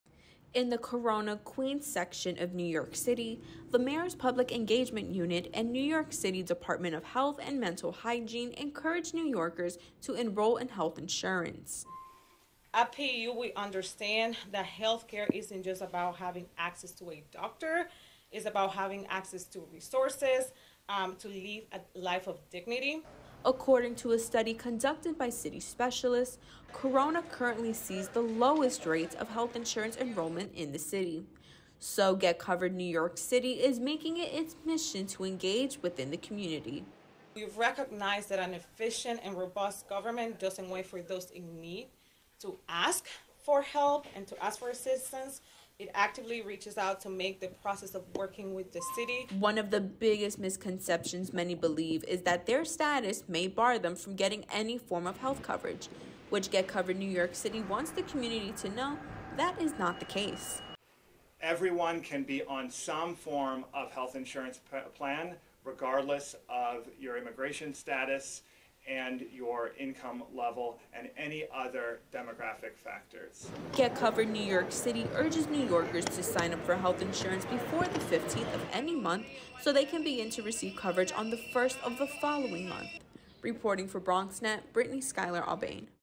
NYC Mayor's Office Encourages New Yorkers to Enroll in Health Coverage
In the Corona, Queens section of New York City, the Mayor’s Public Engagement Unit and NYC Dept of Health and Mental Hygiene encourage New Yorkers to enroll in health insurance.

Reporter: Brittany Skyler Aubain